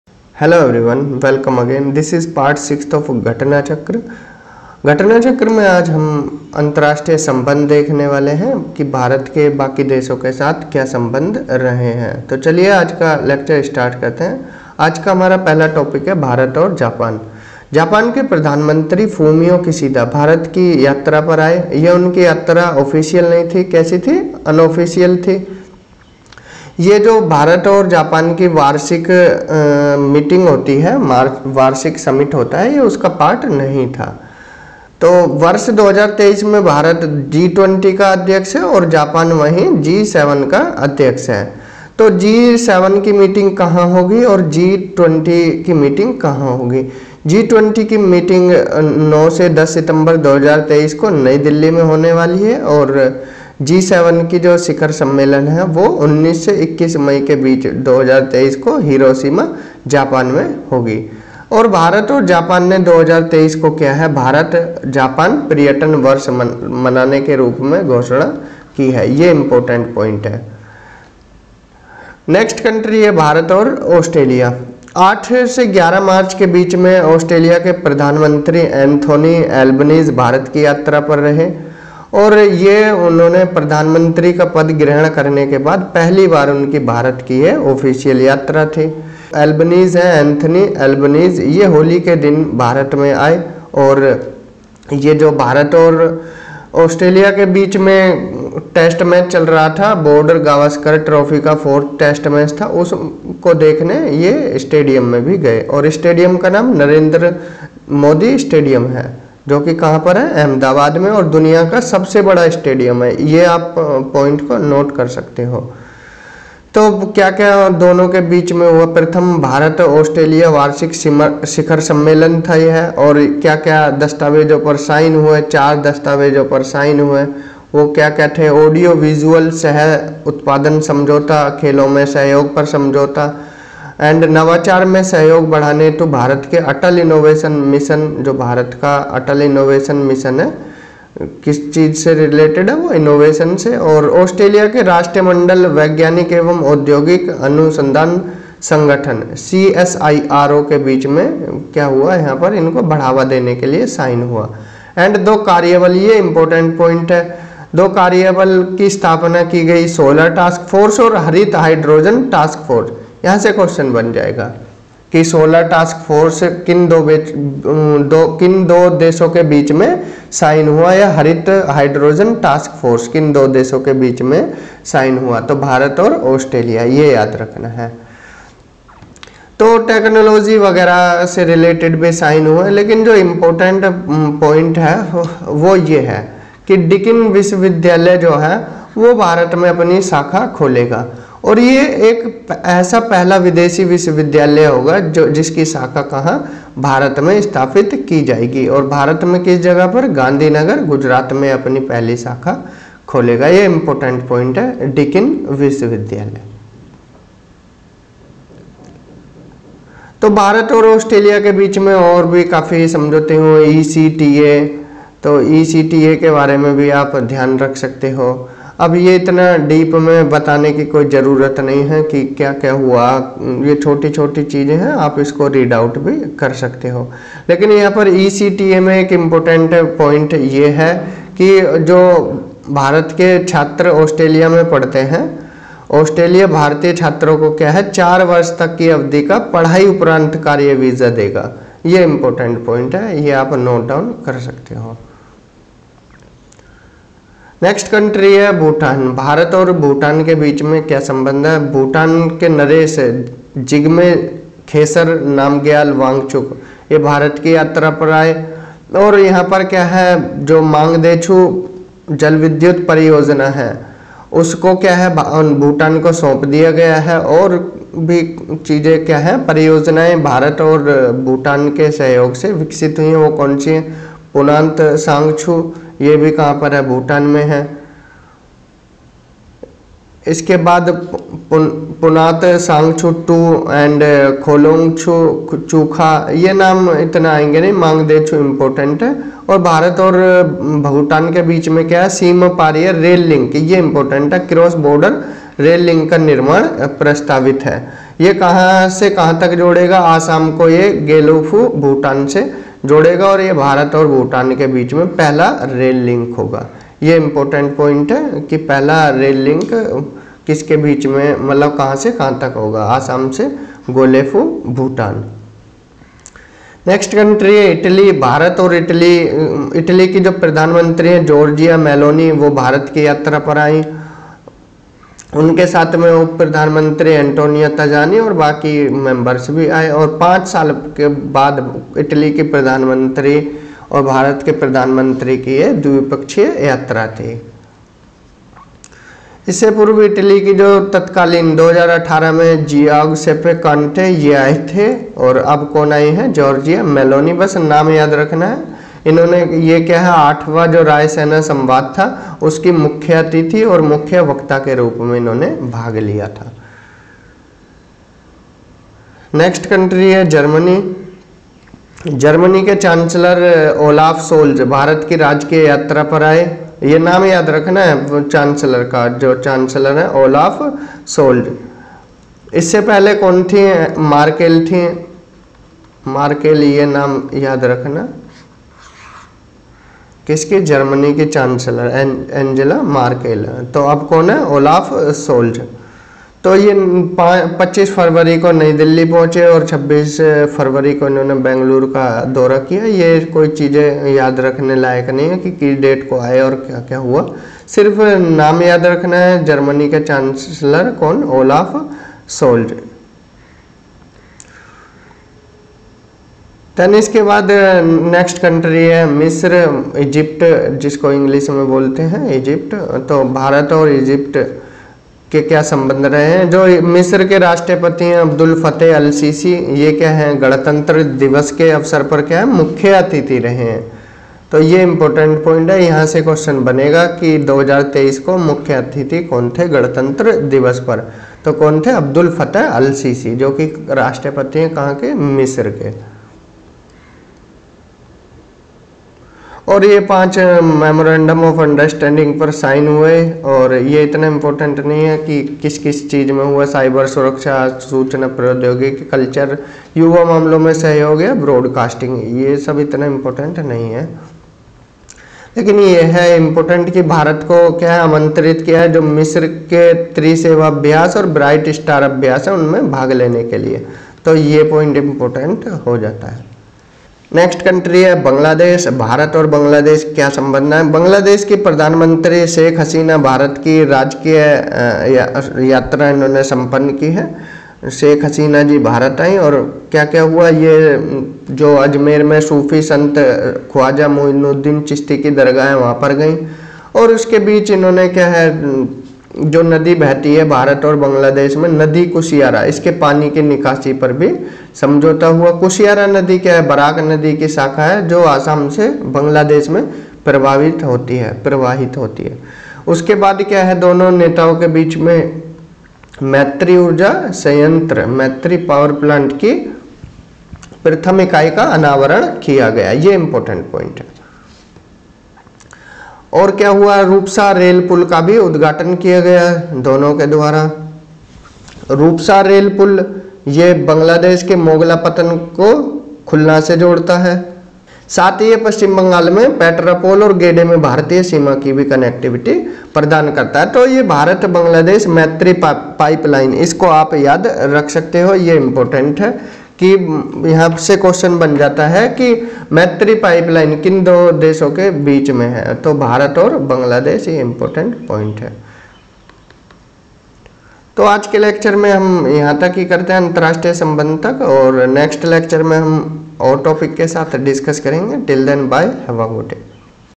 1 Year Current Affairs I Ghatna Chakra I Part-6 I घटना चक्र I Target UP PCS I
Welcome to Study with Rohit Gautam YouTube Channel.
In this series we are going to discuss One Year Current Affair (May 2022 to April 2023).

Instagram: Study with Rohit Gautam